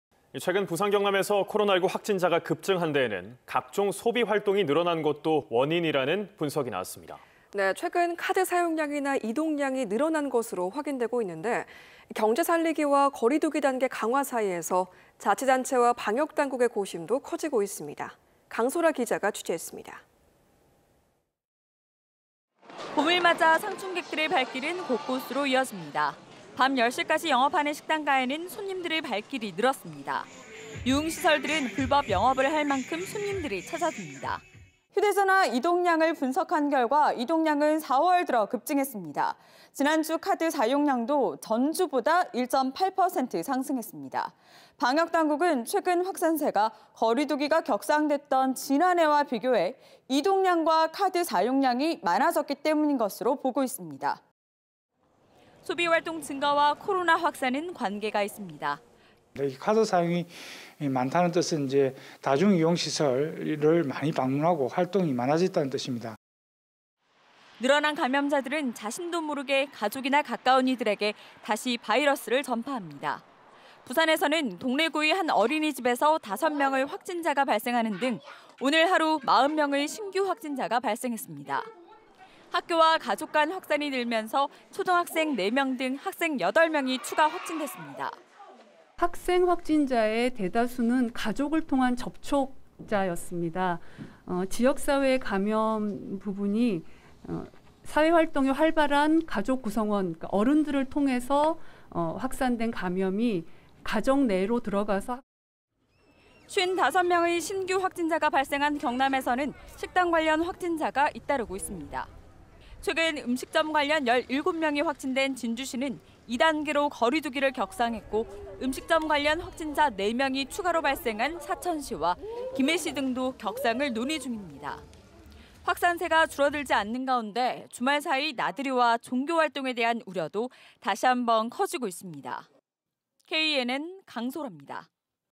이동과 카드사용 급증 4차 대확산 시작?
{앵커:
최근 부산경남에서 코로나19 확진자가 급증한 데에는 각종 소비 활동이
늘어난 것도 원인이라는 분석이 나왔습니다.

최근 카드 사용량이나 이동량이 늘어난 것이 확인되고 있는데,
경제 살리기와 거리두기 단계 강화 사이에서 자치단체와 방역당국의 고심도 커지고 있습니다.

강소라 기자가 취재했습니다.}

{리포트}

봄을 맞아 상춘객들의 발길은 곳곳으로 이어집니다.

밤 10시까지 영업하는 식당가에는 손님들의 발길이 늘었습니다.

유흥시설들은 불법 영업을 할만큼 손님들이 찾아듭니다.

"휴대전화 이동량을 분석한 결과 이동량은 4월 들어 급증했습니다.
지난주 카드 사용량도 전 주보다 1.8% 상승했습니다."

방역당국은 최근 확산세가 거리두기가 격상됐던 지난해와 비교해 이동량과
카드 사용량이 많아졌기 때문인 것으로 보고 있습니다."

소비활동 증가와 코로나 확산은 관계가 있습니다.

{신종우 경남도 복지보건국장장
"카드사용이 많다는 뜻은 다중이용시설을 많이 방문하고
활동이 많아졌다는 뜻입니다."}

늘어난 감염자들은 자신도 모르게 가족이나 가까운 이들에게 다시 바이러스를 전파합니다.

부산에서는 동래구의 한 어린이집에서 5명의 확진자가 발생하는등
오늘 하루40명의 신규 확진자가 발생했습니다.

학교와 가족간 확산이 늘면서 초등학생 4명 등 학생 8명이 추가 확진됐습니다.

{이소라 부산시 시민방역추진단장
"(초등학생 등) 학생 확진자는 대다수 가족을 통한 접촉자였습니다.
지역사회의 감염 대부분이 사회 활동이 활발한 가족 구성원, 어른들을 통해서 확산된 감염이 가정 내로 들어가서"}

55명의 신규 확진자가 발생한 경남에서는 식당 관련 확진자가 잇따르고 있습니다.

최근 음식점 관련 17명이 확진된 진주시는 2단계로 거리두기를
격상했고, 음식점 관련 확진자 4명이 추가로 발생한 사천시와 김해시 등도 격상을 논의중입니다.

확산세가 줄어들지 않는 가운데 주말 사이 나들이와 종교활동에 대한 우려도 다시 한번 커지고 있습니다.

knn강소라입니다.